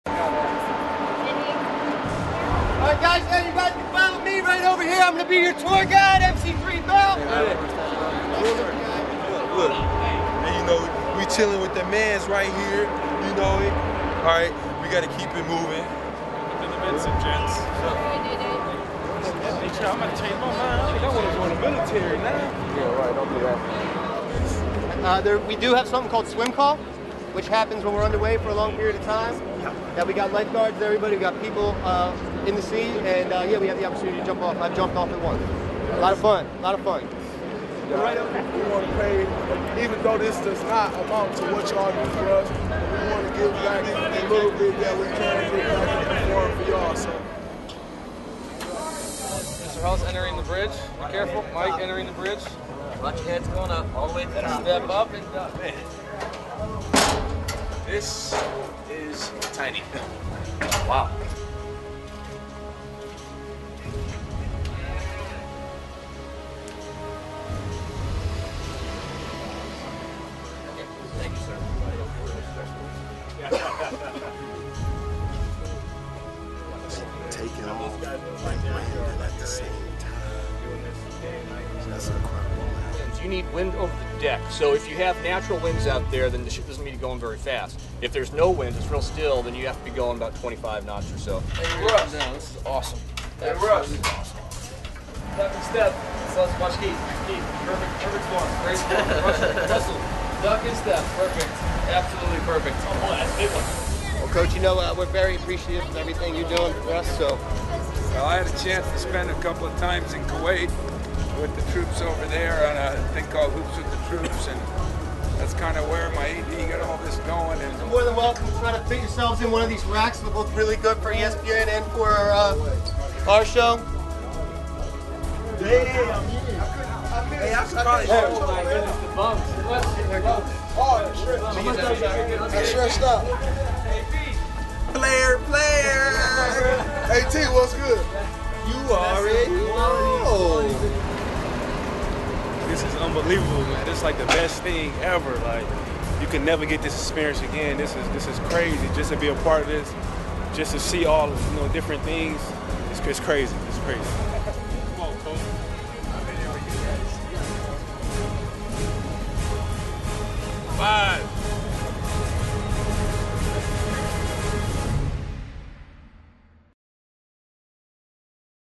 Spartans Visit USS Carl Vinson
The Michigan State Spartan basketball team toured the USS Carl Vinson on Wednesday afternoon. The Spartans will play North Carolina on Nov. 11 in the Quicken Loans Carrier Classic.